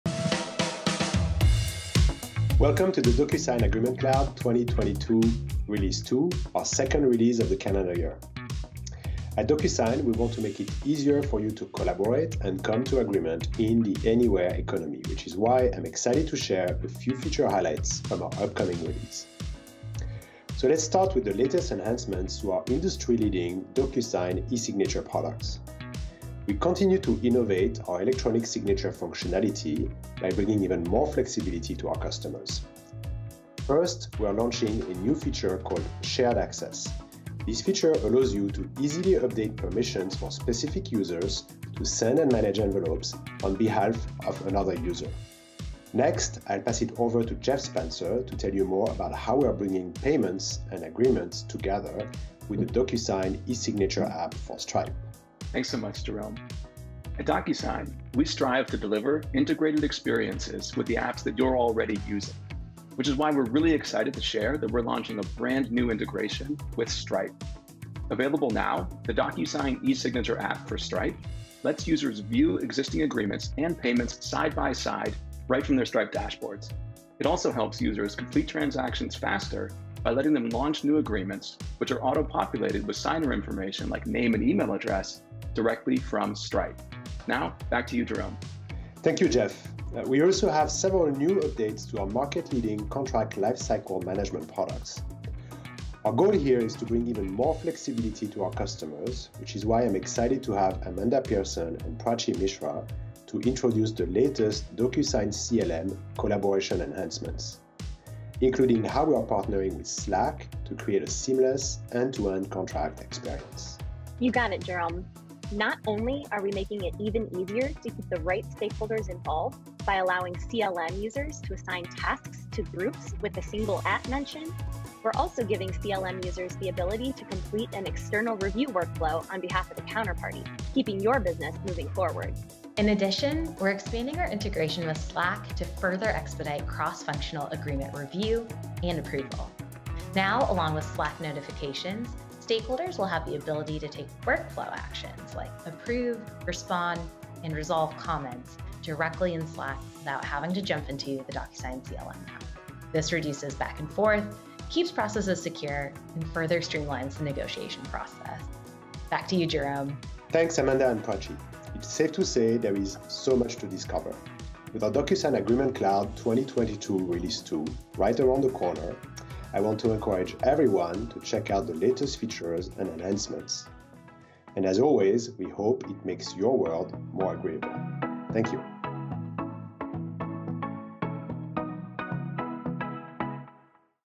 New: The DocuSign Agreement Cloud: 2022 Release 2
We're unveiling exciting new product capabilities to help your organizations enable new ways of living and working in the anywhere economy. Watch and listen to Chief Product Officer, Jerome Levadoux, introduce you to a few of them.

Included in this year's Release 2:

New integrations and features in DocuSign eSignature
- Shared Access allows a user to be granted permissions to send or manage envelopes on another user's behalf
- Agreement Actions enhancements enable users to configure workflows that, upon completion of an initial agreement, automatically trigger a second agreement
- DocuSign eSignature App for Stripe is a new integration that lets account, finance and support teams view eSignature agreements and Stripe payments side-by-side, and launch new agreements right from their Stripe dashboards

New integrations and features in DocuSign CLM and Gen
- Users can assign tasks to a group with a single @-mention, as well as complete external review on behalf of a counterparty to keep workflows moving forward
- CLM Obligations for Salesforce allows customer-facing teams within an organization to view the latest obligations per account, right from Salesforce in a Lightning component
- DocuSign Gen for Salesforce Subscription Management enables users to easily generate accurate transaction receipts, invoices and account statements related to existing or evergreen subscriptions